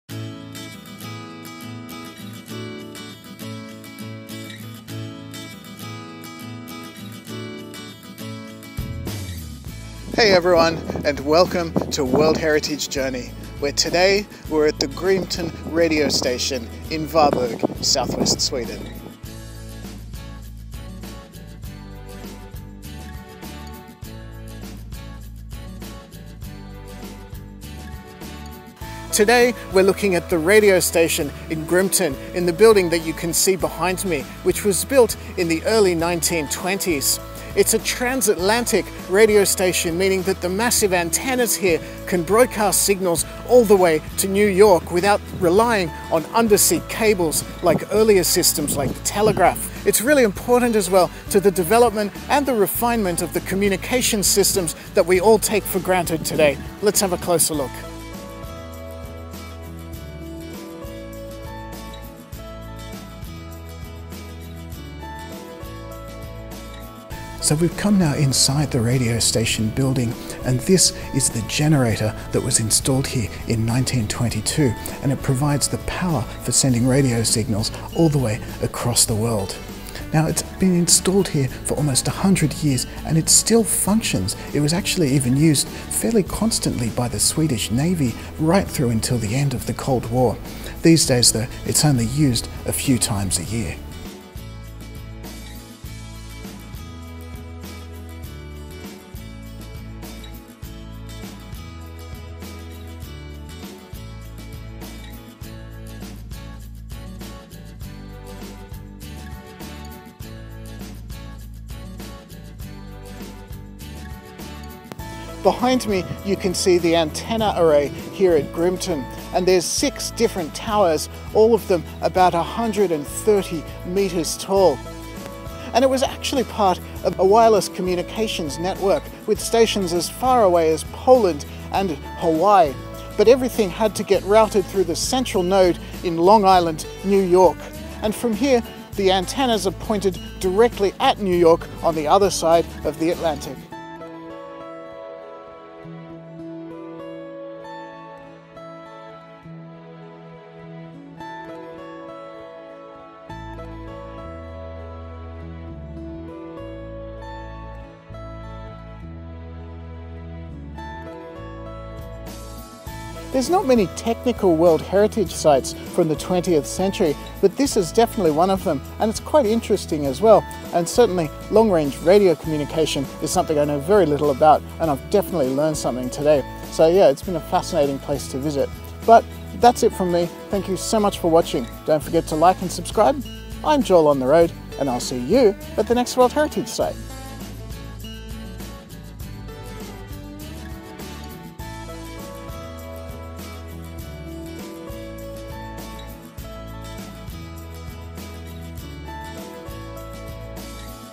Grimeton Radio Station - UNESCO World Heritage Site
The UNESCO World Heritage site Grimeton Radio Station in south-western Sweden is an early example of transatlantic radio communications. The enormous antennae and massive generator had enough power to send longwave radio signals directly across the Atlantic to the central processing node in New York. Despite being built in the 1920s, it's actually still functional though is only used on a few special days each year. Let's check it out!